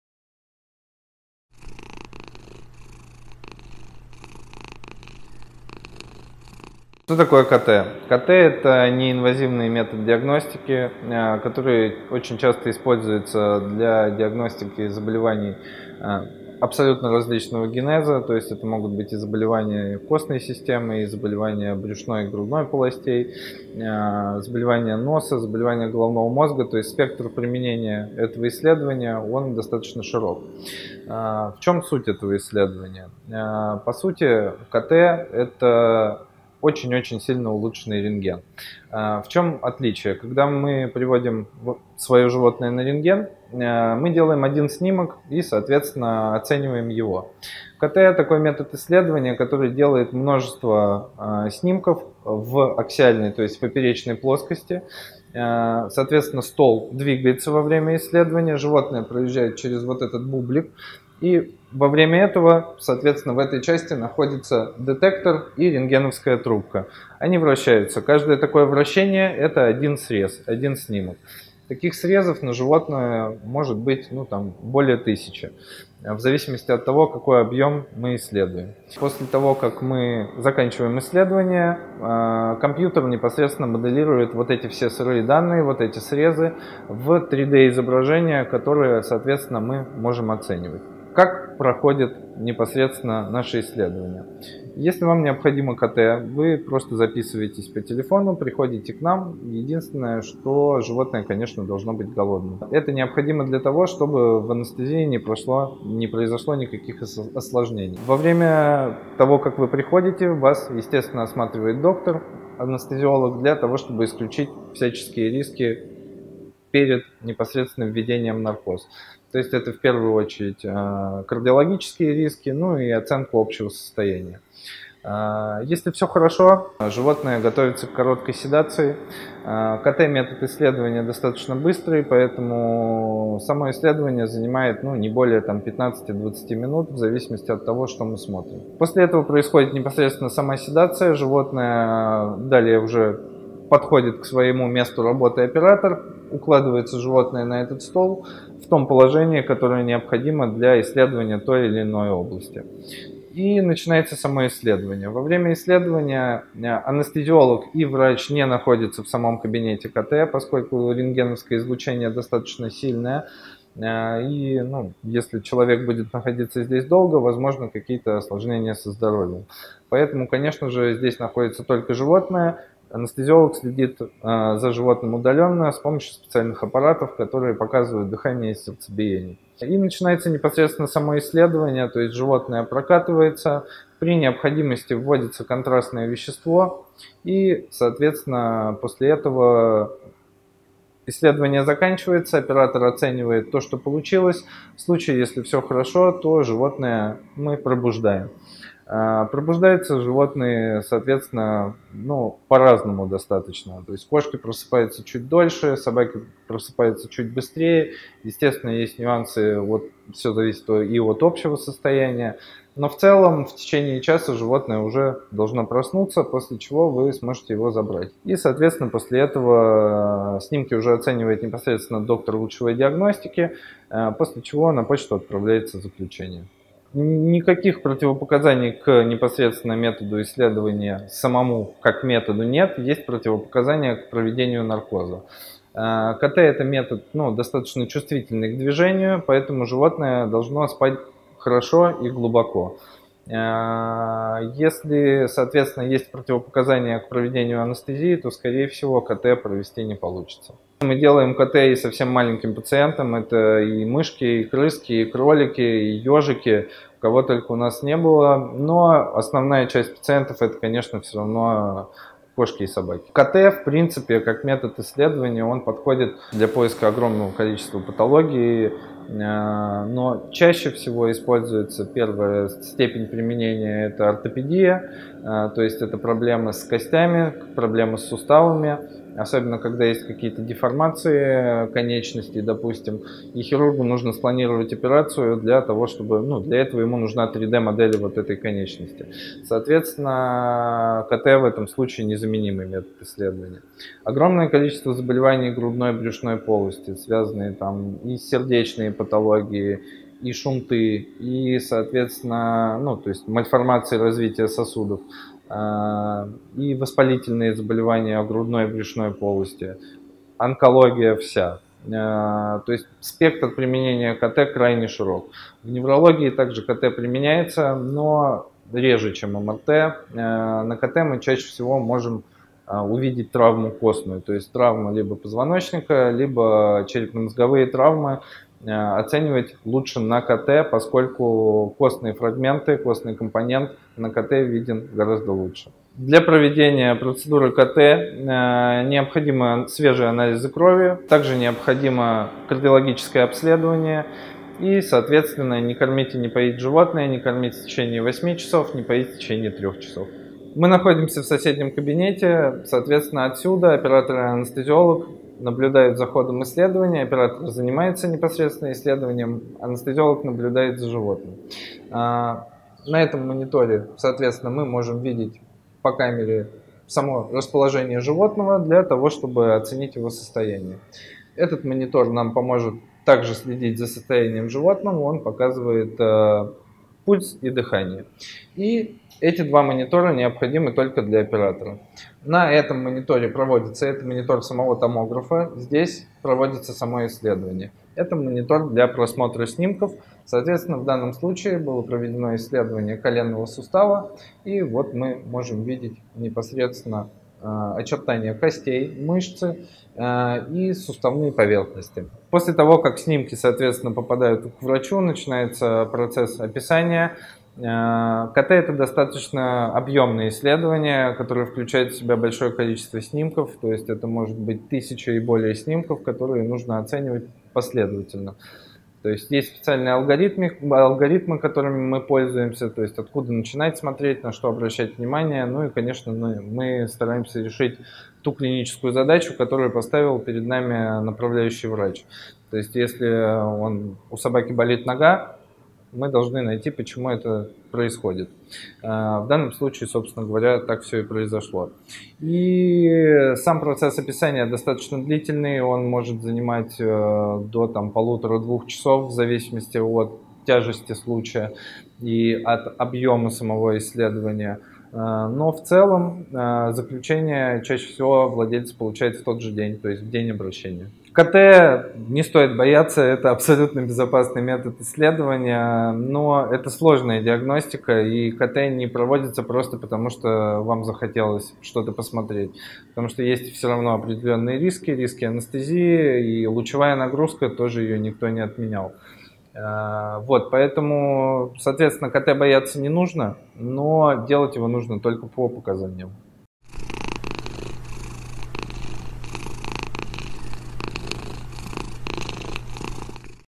КТ для питомца: что нужно знать владельцу
КТ, или компьютерная томография -- звучит пугающе, особенно когда врач направил на это исследование любимого питомца. Как он это переживёт, не опасно ли это? Не нужно бояться, надо разобраться!

Что такое КТ, как подготовить к нему животное, как проходит само исследование и зачем оно нужно вашему питомцу? Ответы на все важные вопросы даст Артём Корешков -- ветеринарный невролог, врач КТ- и МРТ-диагностики, главный врач "Европейского центра рентгенодиагностики для животных".

Запись видео проведена при поддержке Грантов Мэра Москвы для социально ориентированных НКО Комитета общественных связей и молодежной политики города Москвы.